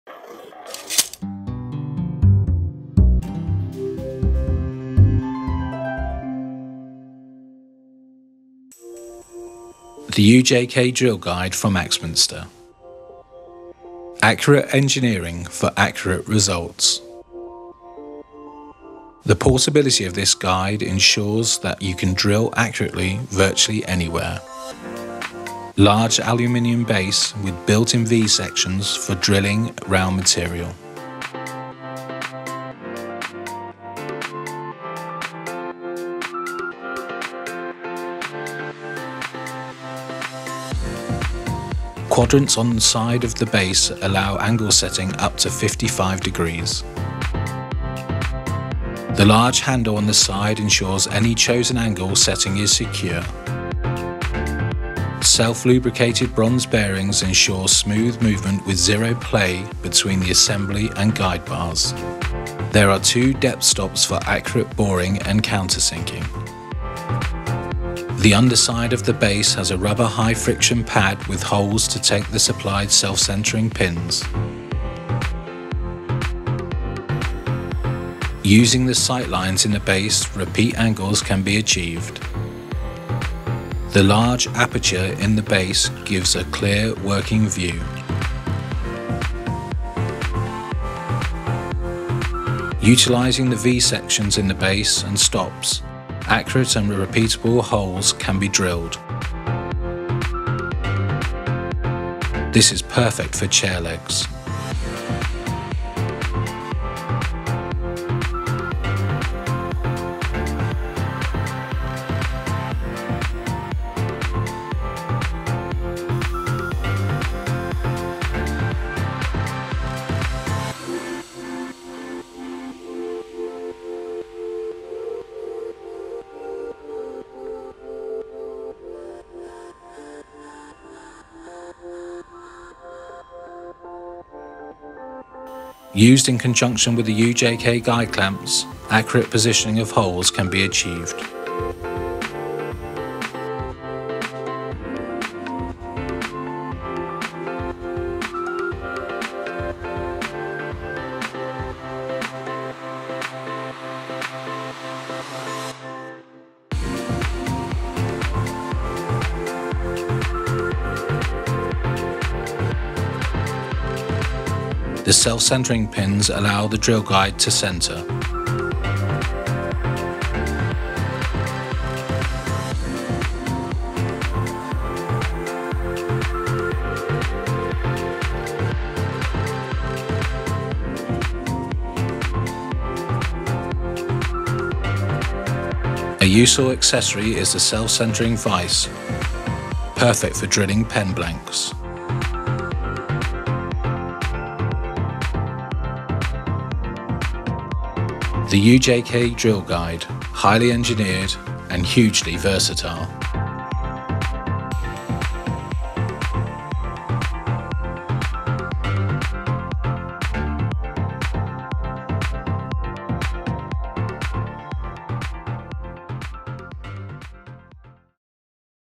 UJK Drill Guide - Product Overview
It is practically impossible to drill at a specific angle freehand. This highly engineered drill guide guarantees precise results. The UJK Technology drill guide is in effect a hugely versatile and portable pillar drill. The portability of this guide ensures that you can drill virtually anywhere. Set at 90° it produces perfectly perpendicular holes. Quadrants on each side of the base allow angle setting at ±55°. A threaded knob on one side and the auxiliary handle on the other ensure any chosen angle setting is secure. The underside of the base has a rubberised high friction pad to help steady the guide. The large aperture in the base gives a clear view of the drilling mark and allows the use of bits up to 60mm diameter. Integral vee-guides mean you can drill round the stock or pipes. The base has holes for fixing the guide in place onto a jig or sub-base.

EBAY STORE